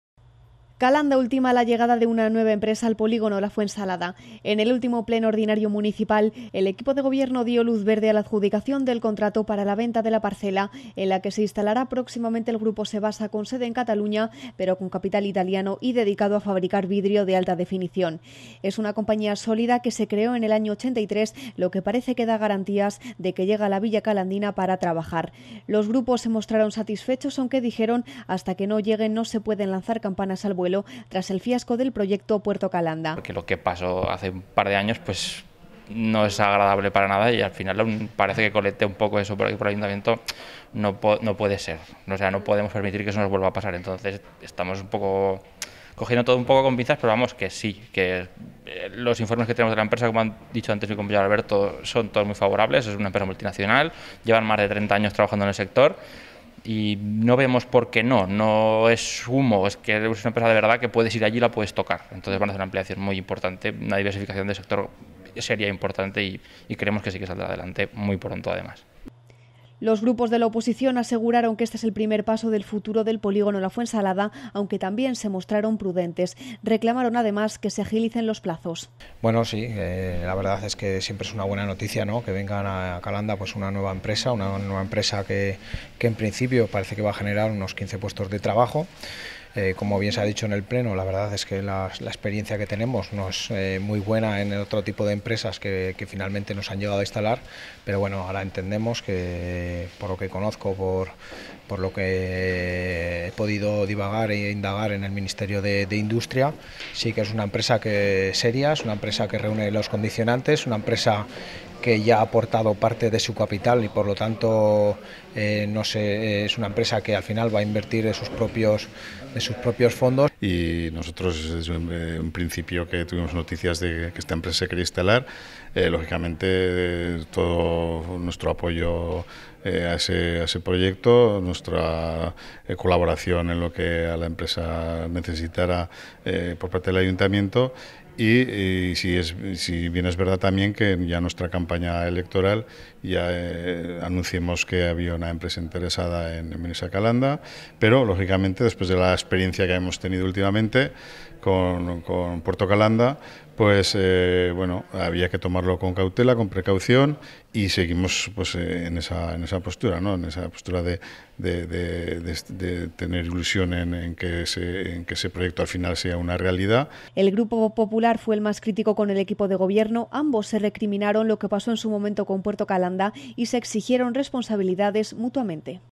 La Comarca.tv - Valoraciones pleno Calanda de 7 noviembre
Valoraciones de los portavoces de los diferentes partidos políticos del ayuntamiento de Calanda tras la sesión de pleno ordinario celebrado en el consistorio con fecha lunes 7 de noviembre de 2016. Para más información se puede ver el pleno íntegro en la página